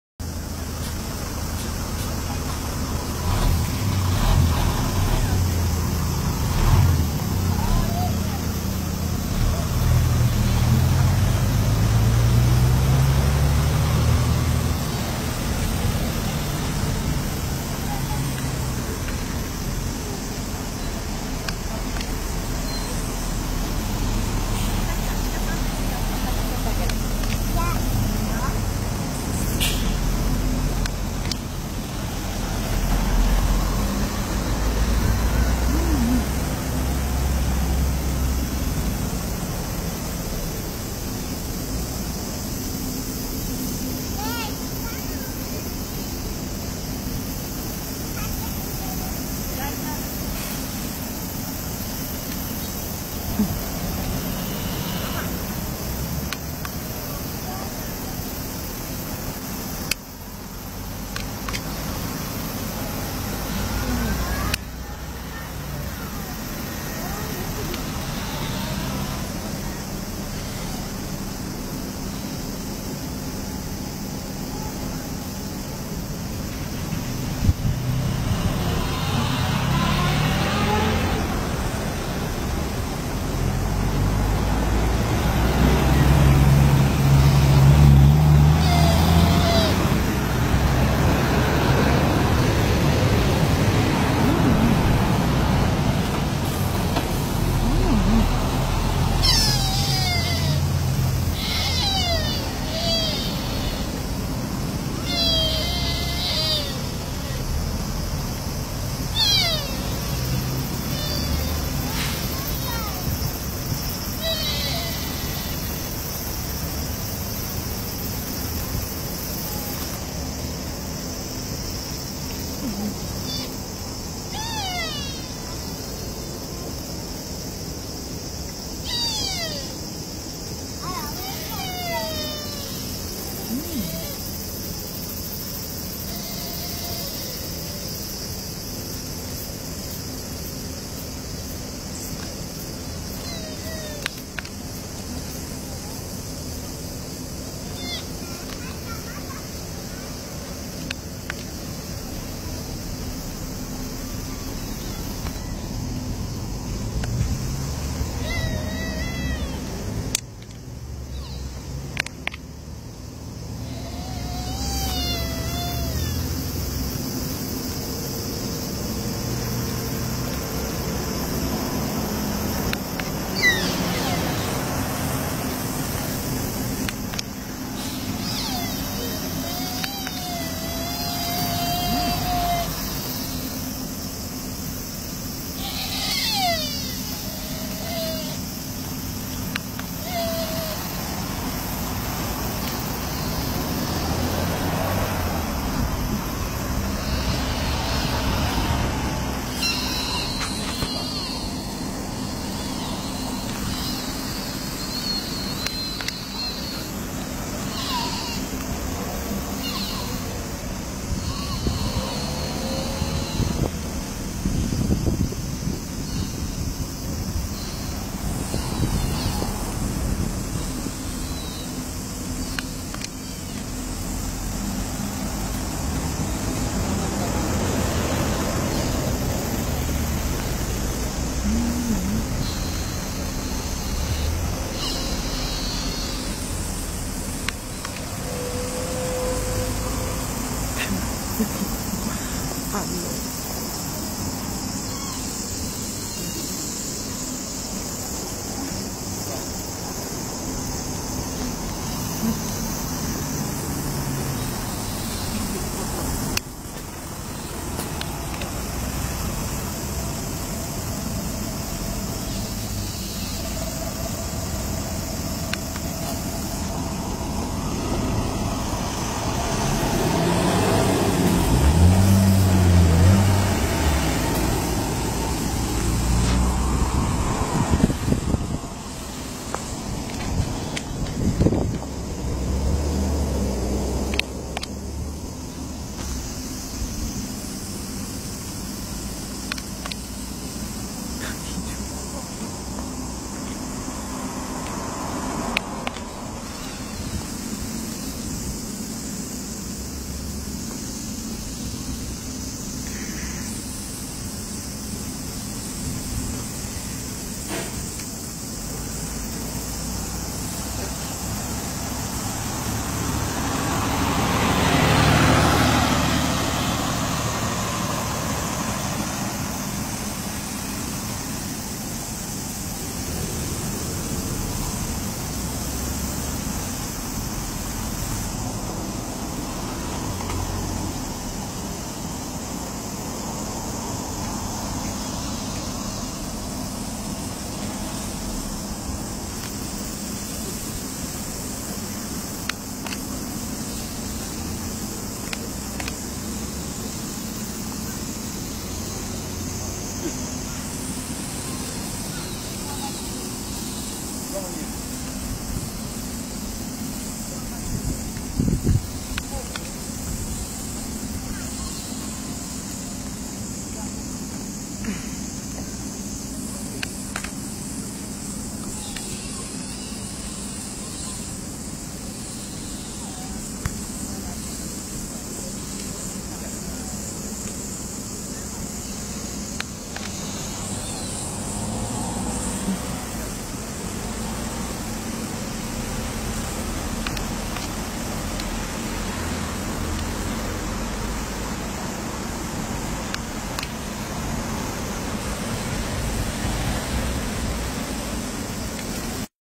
Cervos Invadem cidade.
Uma manada de cervos invadem uma cidade na asia.